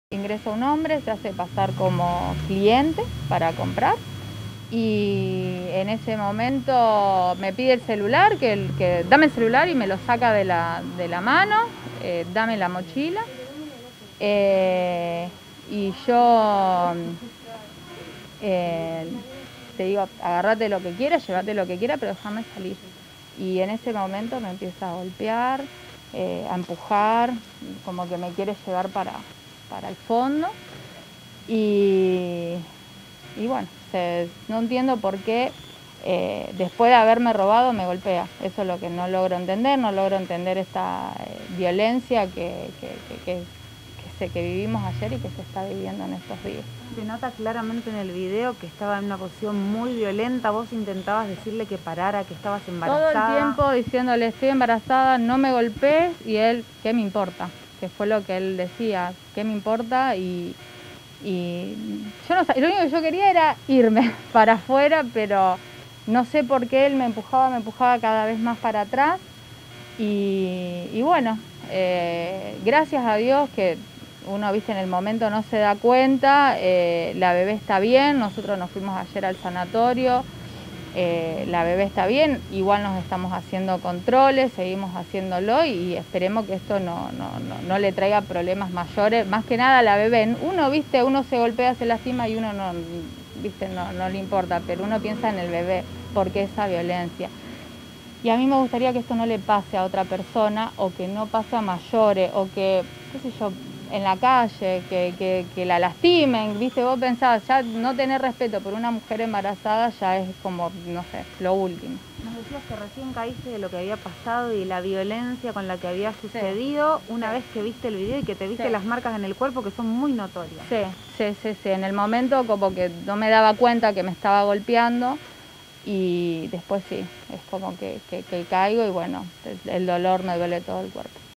Video. Golpeó y robó a una joven embarazada en una tienda de ropa. Villa Gdor Gálvez.
OCURRIÓ EN UNA TIENDA DE ROPA EN PLENO MEDIODÍA
ROCÍO BENÍTEZ – DUEÑA DEL LOCAL Y VÍCTIMA

Este jueves en horas del mediodía, en un local ubicado en pleno centro de la ciudad de Villa Gobernador Gálvez, ocurrió un fuerte hecho delictivo en el que un hombre agredió físicamente a una mujer embarazada.
Allí, un sujeto ingresó a la tienda de ropa para niños haciéndose pasar por un cliente, al momento de abonar la supuesta compra, el hombre se abalanzó contra la víctima para arrebatarle su teléfono celular y su cartera. El momento quedó grabado por la cámara de seguridad del local.
No solo comete el robo, sino que agrede físicamente a la mujer y llega a tomarla del cuello. Tras hacerla caer, la volvió a empujar en reiteradas ocasiones mientras ella le repetía: "Estoy embarazada".
Vecinos aseguran que el mismo hombre entro a robar a otros locales ese día.